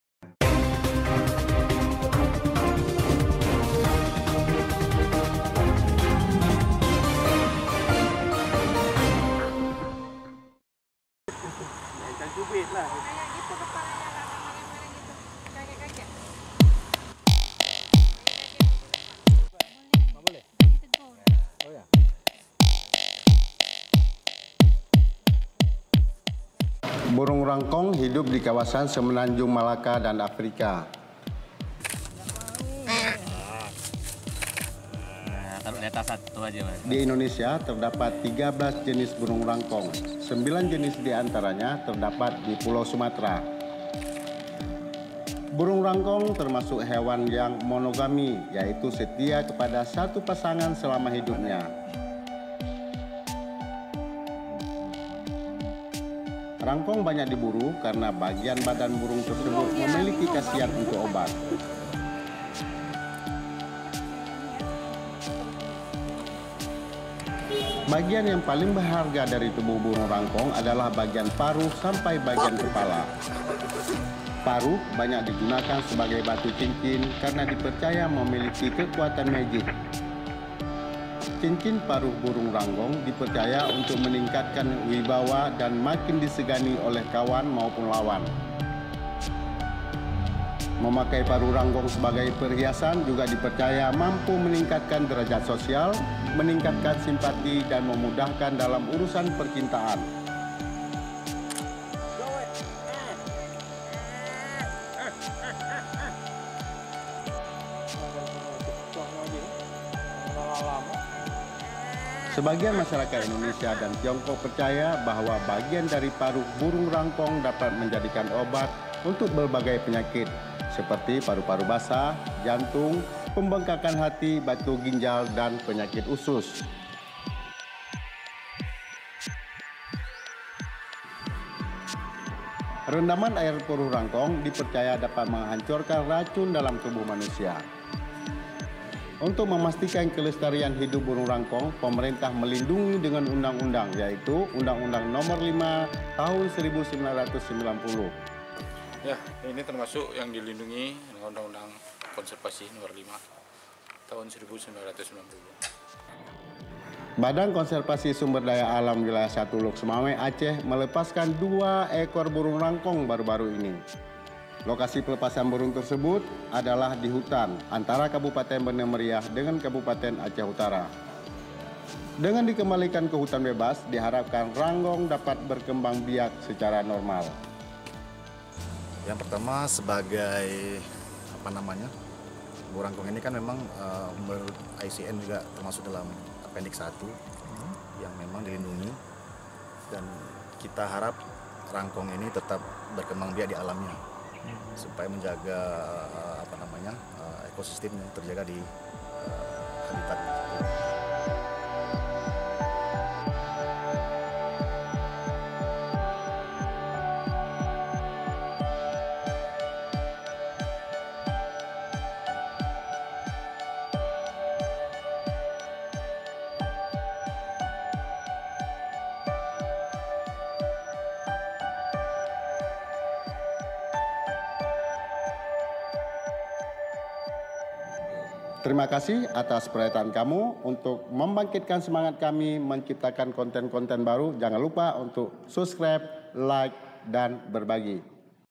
Diburu Untuk Magic
BURUNG RANGKONG TERGOLONG BURUNG MONOGAMI, YAITU MEMILIKI SATU PASANGAN SELAMA HIDUPNYA//

BAGIAN SEBAGIAN WARGA DI  INDONESIA/ BURUNG RANGKONG DIPERCAYA SEBAGAI TITISAN DEWA//

PARUHNYA UNTUK OBAT DAN MAGIC.